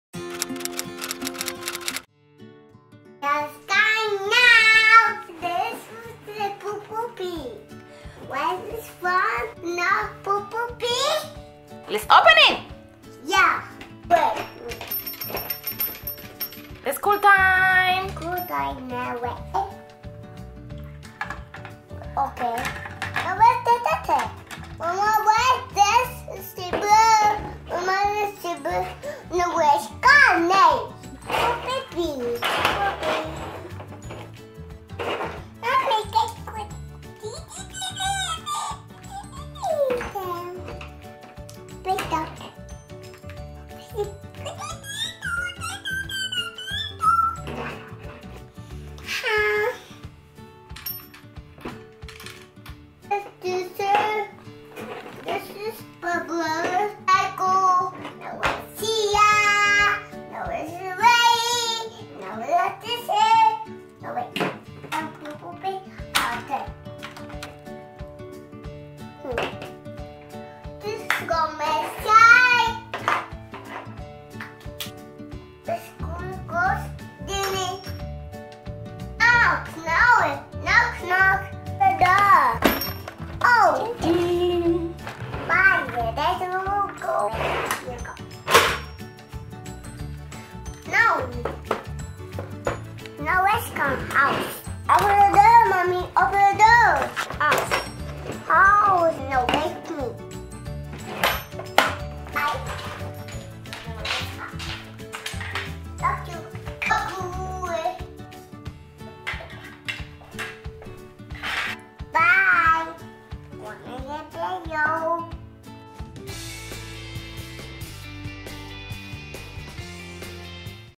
Unboxing Peppa Pig's School Playset with Zoe Zebra & Madame Gazelle
Daniela is having fun with a Peppa pig playset!!! Watch all the cool accessories and Daniela's reactions!!